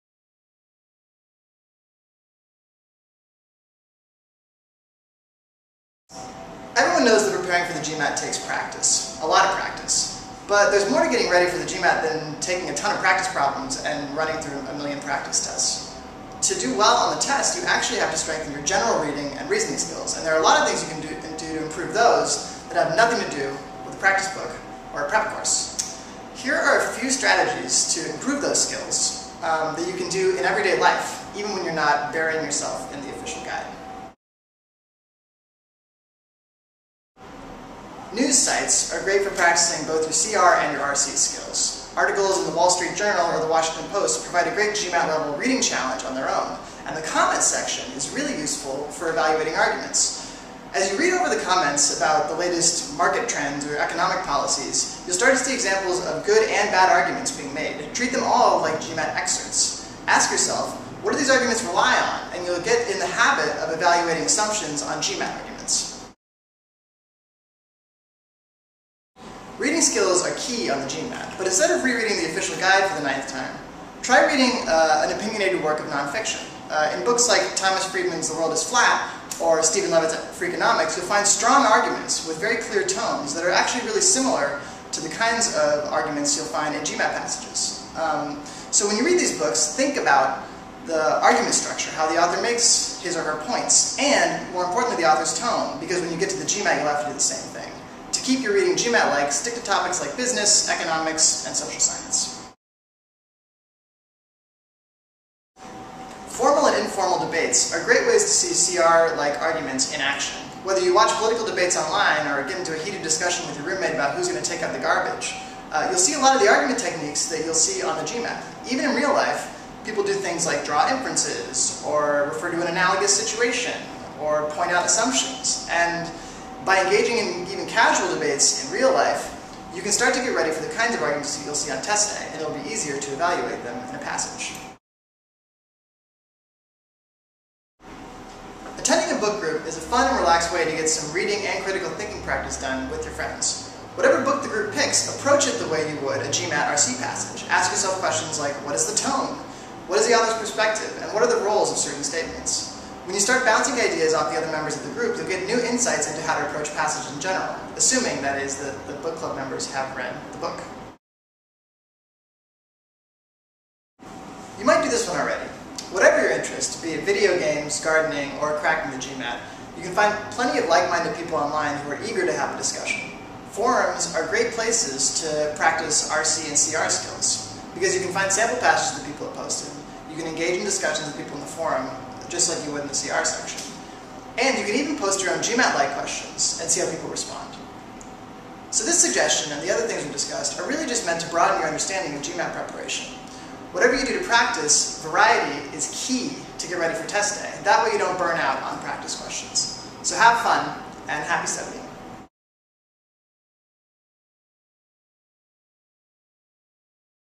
GMAT How To's: How to practice when you're not doing practice problems
5 tips for sharpening your GMAT skills even when you're not doing practice problems or exploring a prep course.